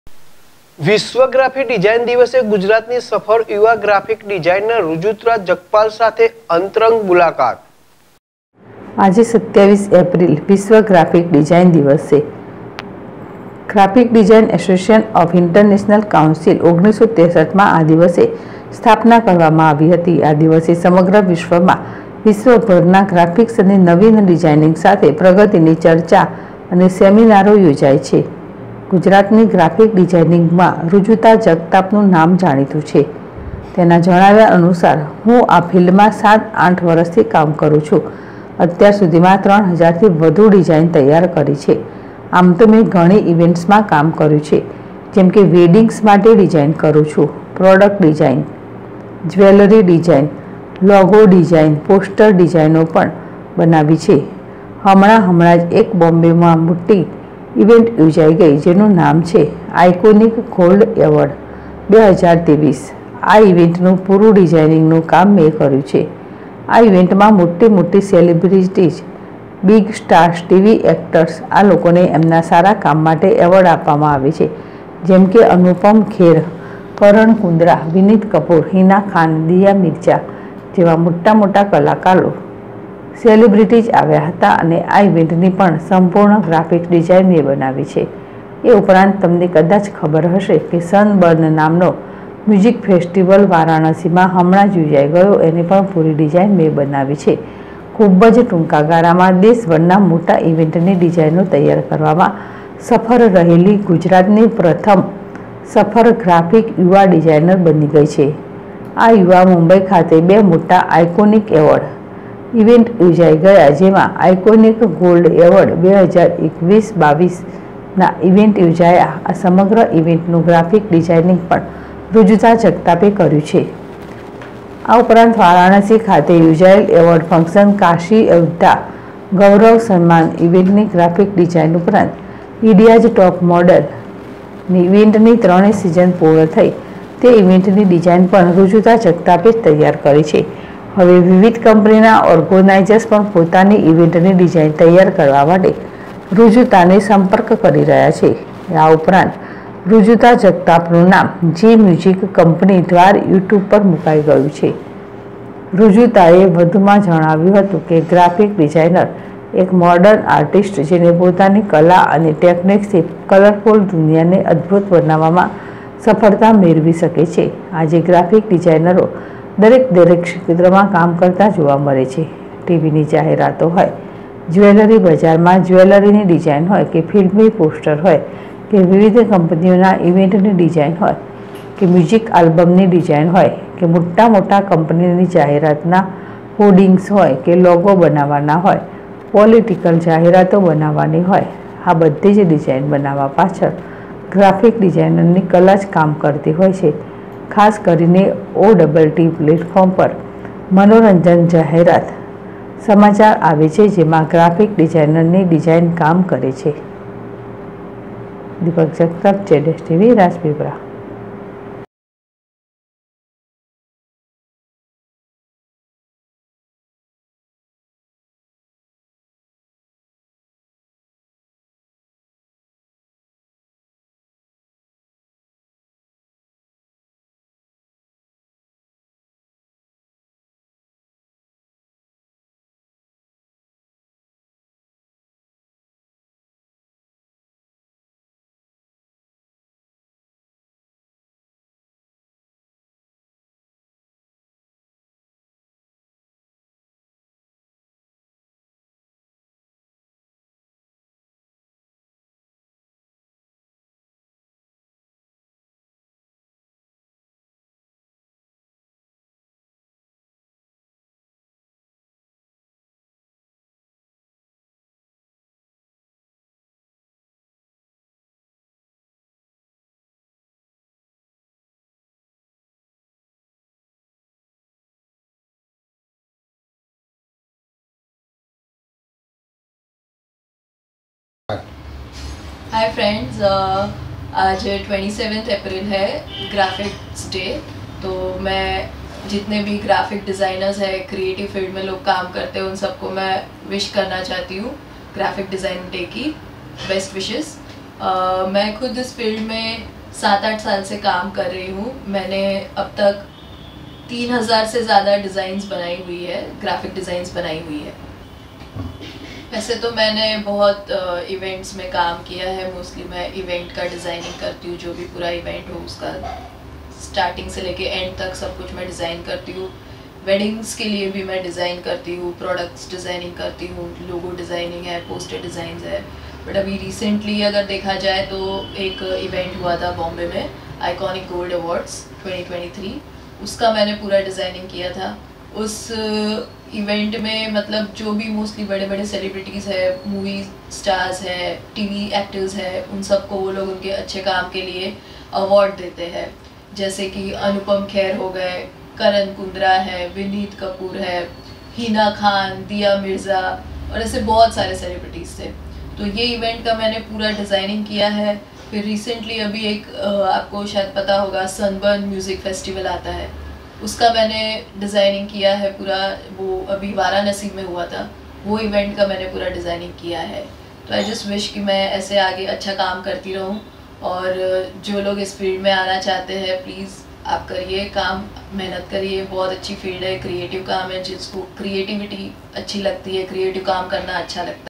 વિશ્વ ગ્રાફિક ડિઝાઇન દિવસે ગુજરાતની સફળ યુવા ગ્રાફિક ડિઝાઇનર ઋજુતા જગતાપ સાથે અંતરંગ મુલાકાત
#, રાજપીપલા વિશ્વ ગ્રાફિક ડિઝાઇન દિવસે ગુજરાતની સફળ યુવા ગ્રાફિક ડિઝાઇનર ઋજુતા જગતાપ સાથે અંતરંગ મુલાકાત